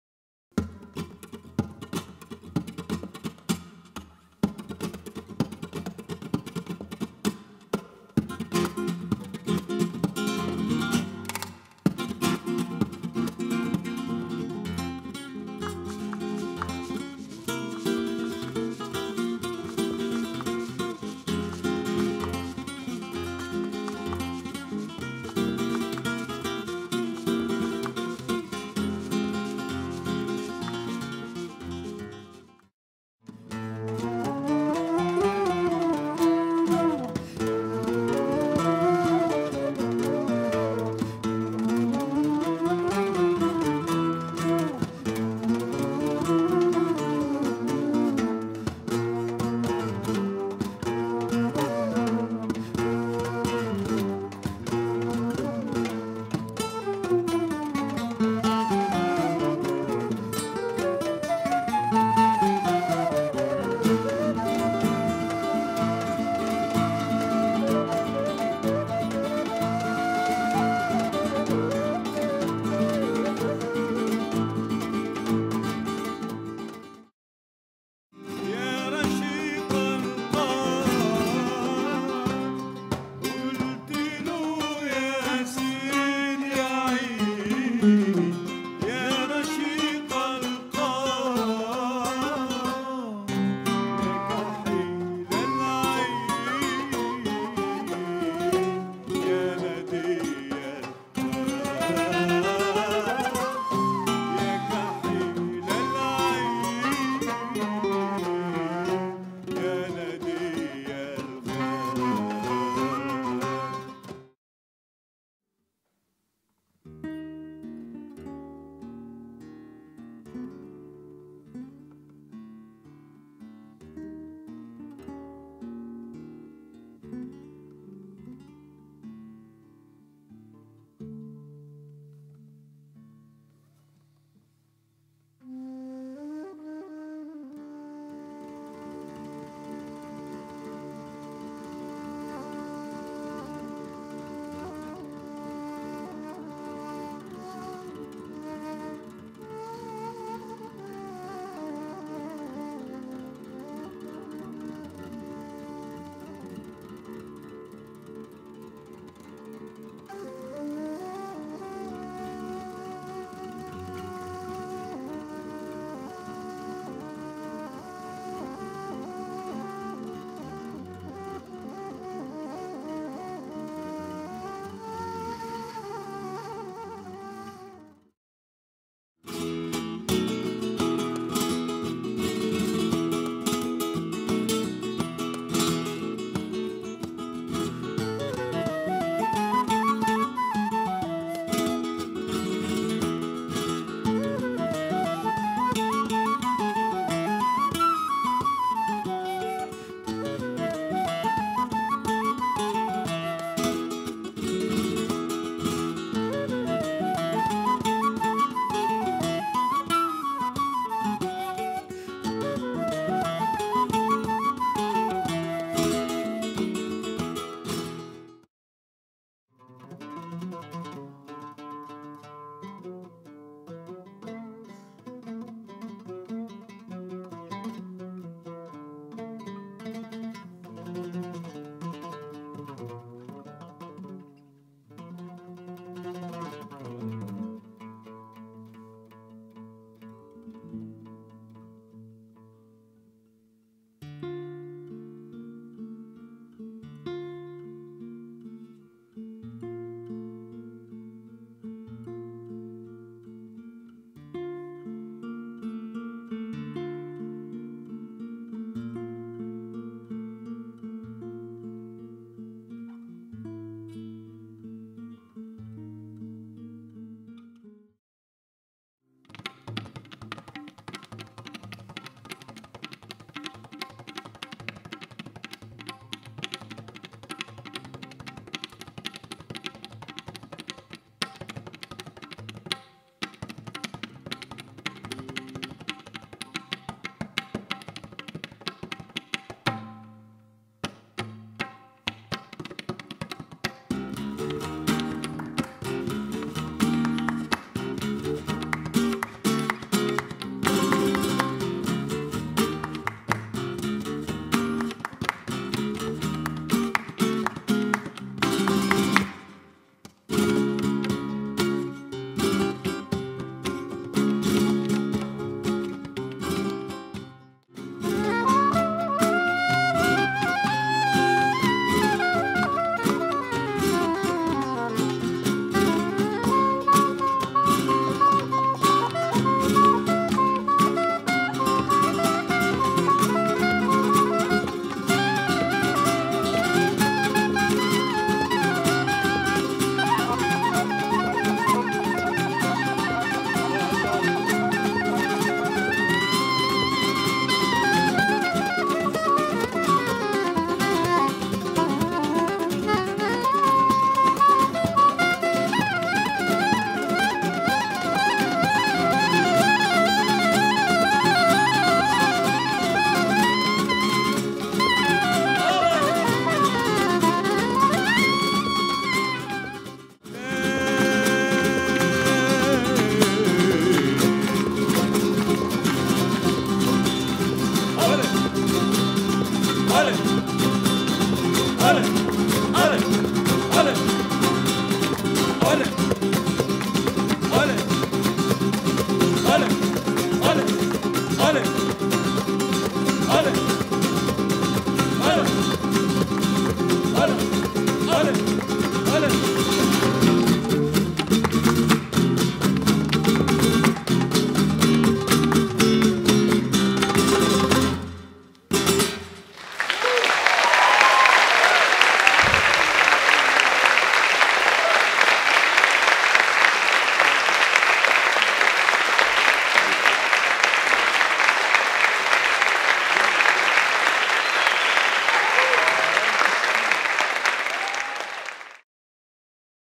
Juan Martín Musica Alhambra 2013
live film of Juan Martín and Musica Alhambra with Abdul Salam Kheir on oud, Louai Alhenawi on ney flute, Paul Fawcus on soprano sax, flute and clarinet and Chris Karan on percussion.